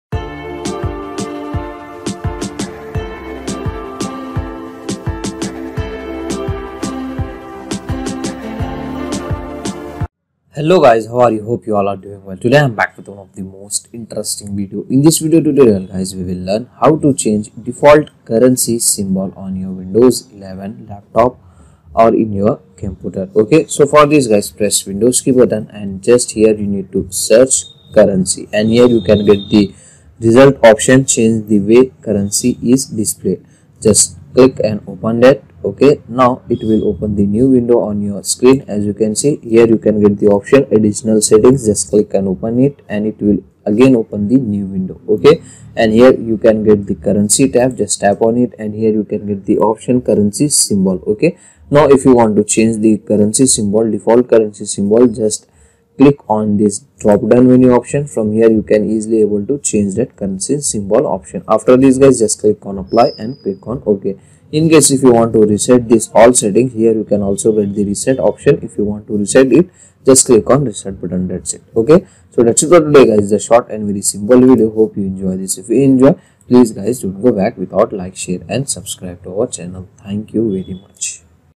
How to Change Default Currency Symbol in Windows 11 on Computer or Laptop
On Windows 11 How to Change Default Currency Symbol
How to Reset Currency Symbol in Windows 11